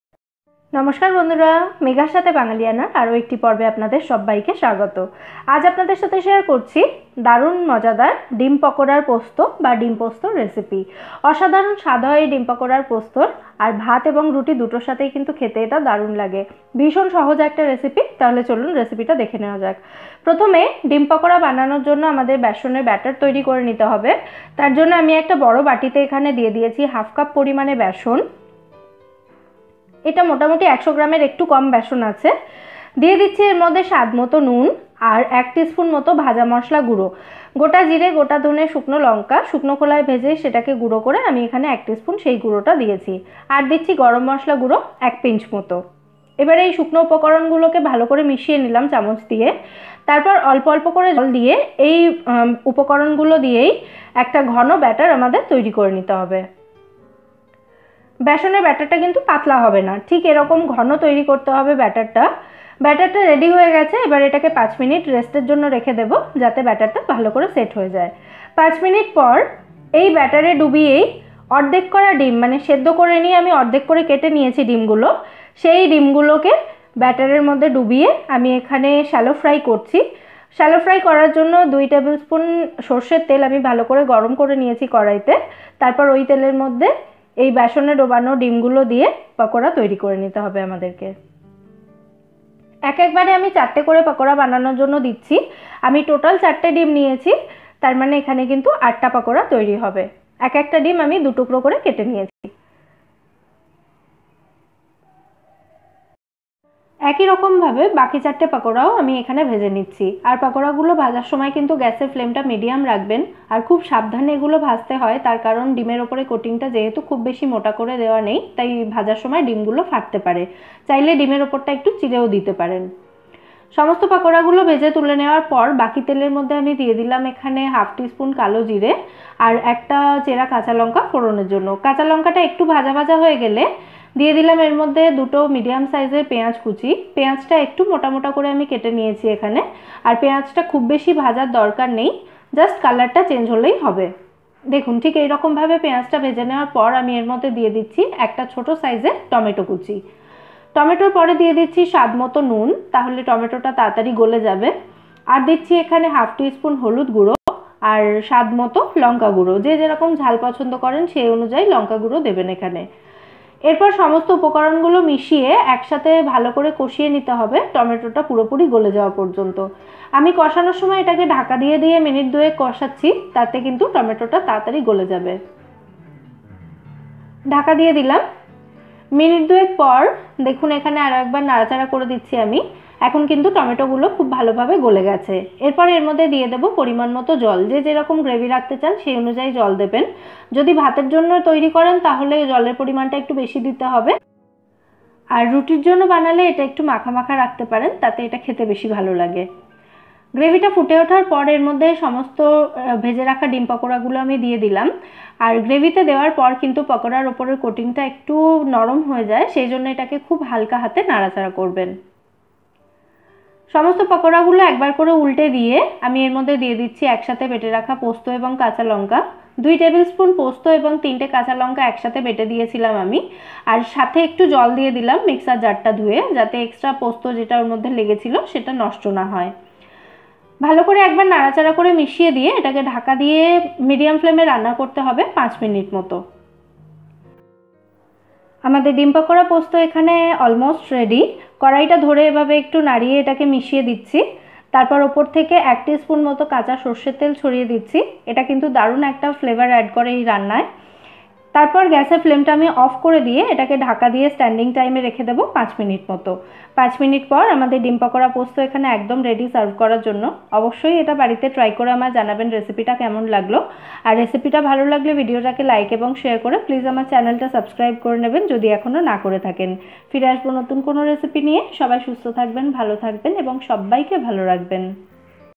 ডিমের একদম নতুন এই রেসিপি জানা থাকলে বারবার এটাই বানাবেন || ডিম পোস্ত || ডিম পকোড়া পোস্ত
Dim pakoda posto

Ingredients
4 eggs
2 onions
1 small tomato
Red chilli powder to taste
2 tbsp poppy seeds
3 green chillis
2tbsp mustard oil
Half tsp nigella seeds
1 green chilli

Half cup besan or gram flour
Salt to taste
Bhaja masala 1tsp
1 pinch garam masala powder